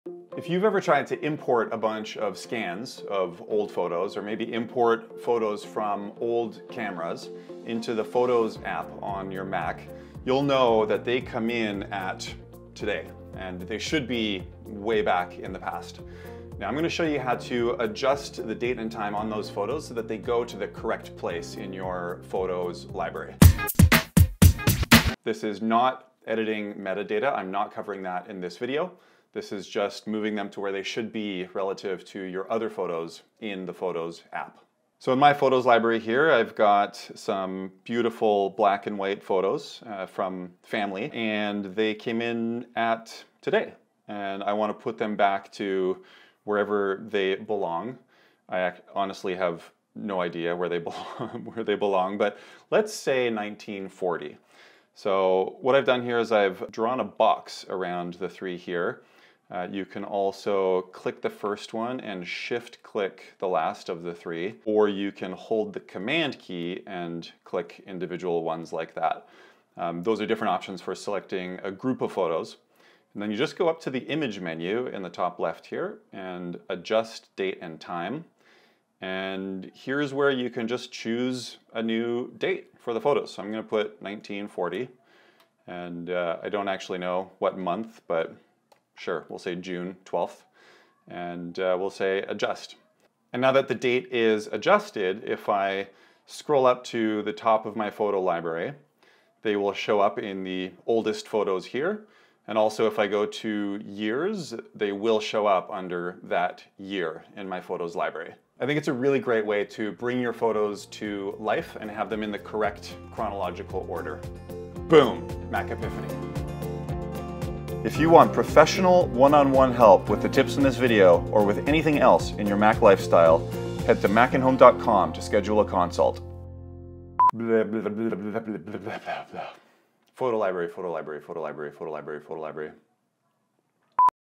How to Fix Photo Dates in Mac Photos App
Importing or scanning a bunch of photos brings them in at today's date. Here's the fix to get those photos to their rightful place in your Photos library!

Special thanks to Andrey Summers and Neuman Mannas of Red Square Collective for the awesome video.